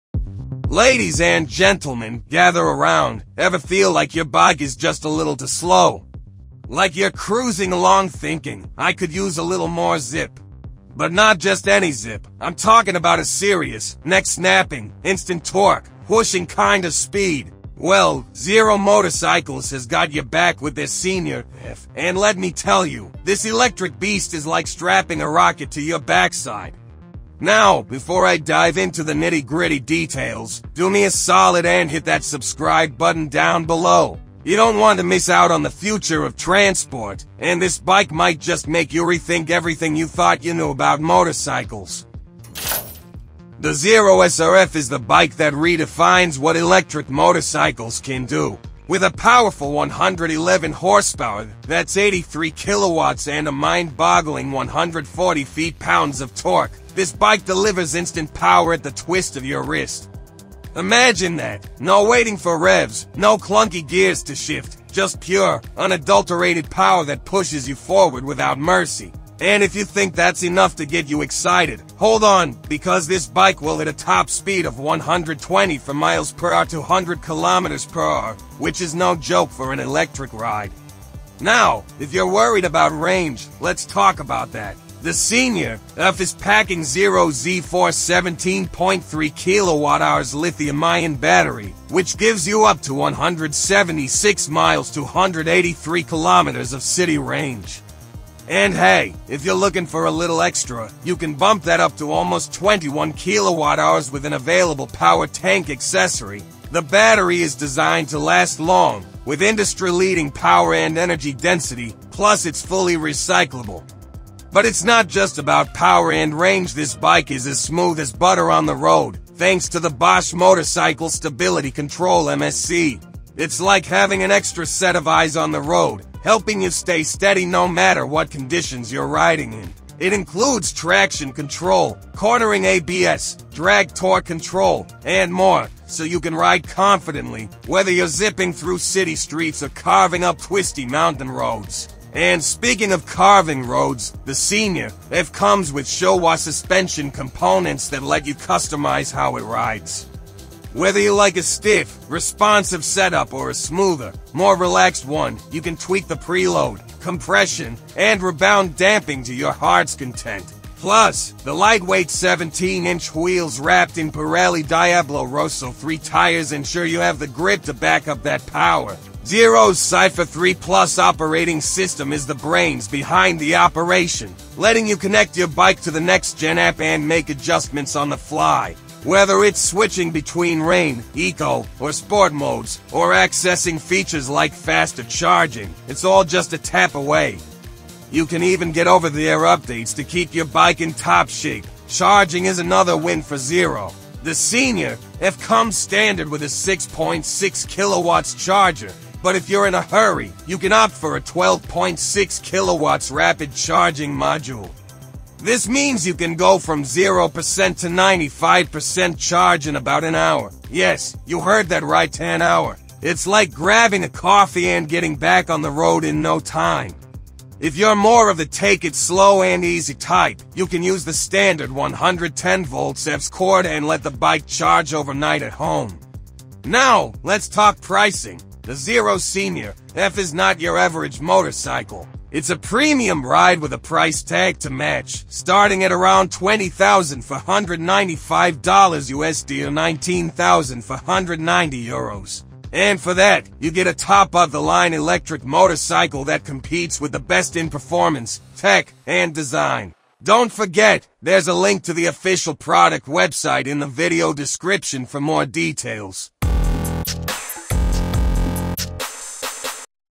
2025 Zero SR/F: The Ultimate Electric Motorcycle with 111 HP and 124 MPH Top Speed
The Zero SR/F is the ultimate electric motorcycle, delivering 111 horsepower and a top speed of 124 mph 🚀. This high-performance bike is perfect for thrill-seekers looking for instant torque and a smooth ride. Powered by Zero’s innovative Z-Force 17.3 kWh lithium-ion battery, it offers up to 176 miles of city range and features an industry-leading rapid charging system ⚡. With advanced tech like Bosch Motorcycle Stability Control and Zero’s Cypher III+ operating system, the SR/F is a game-changer for electric motorcycling 🔥. For those in a rush, the optional 12.6 kW Rapid Charging Module can juice up the bike from 0% to 95% in about an hour!

If you’re a speed enthusiast and eco-conscious rider, the SR/F is for you.